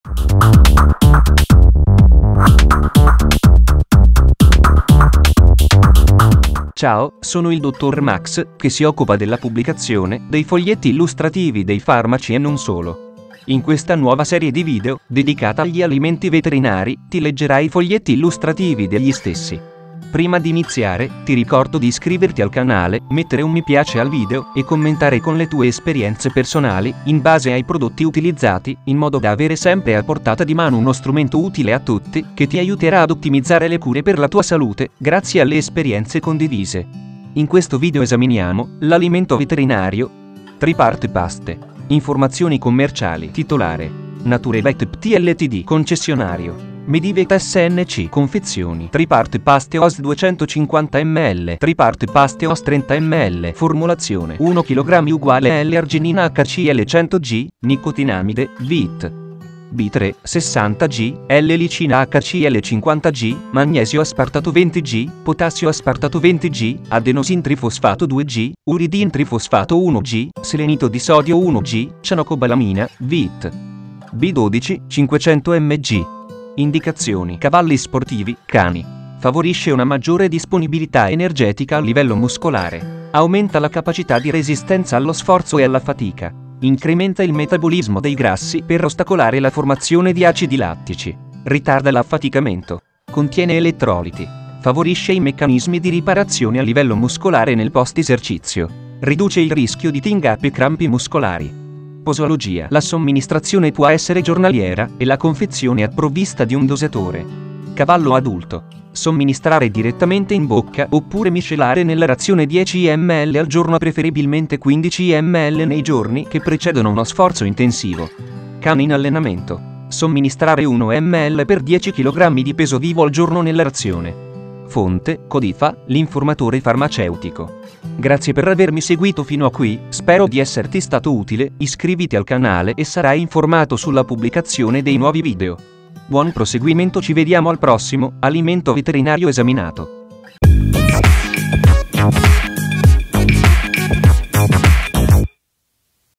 🍪 TRIPART PASTE Alimento Veterinario a cosa serve ? - Foglietto Illustrativo Salute
❤️ Info Salute: Abbonati a questo canale per accedere ai vantaggi

Abbiamo letto il Foglietto Illustrativo e parlato dell' Alimento Veterinario: TRIPART PASTE

Capitoli ⏱
0:00 Introduzione
0:06 Presentazione
0:40 Alimento Veterinario TRIPART PASTE
1:01 A cosa serve ?

TRIPART PASTE Alimento Veterinario